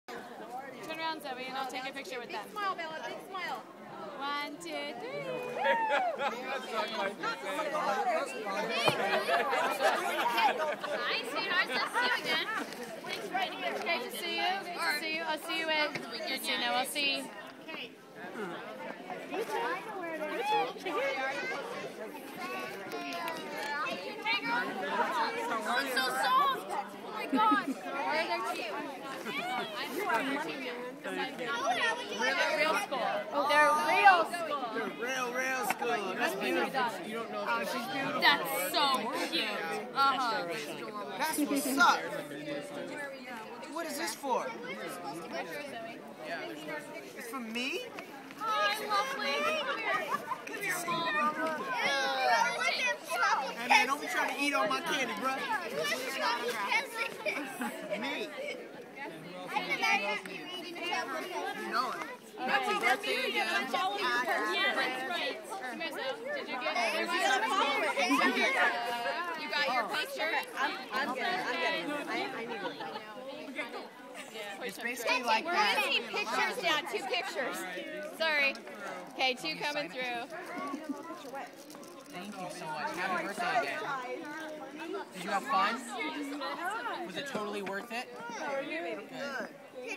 Meting the Shake it up cast
I met them after a taping of Embarrass it up 2/14/12
Valentines Day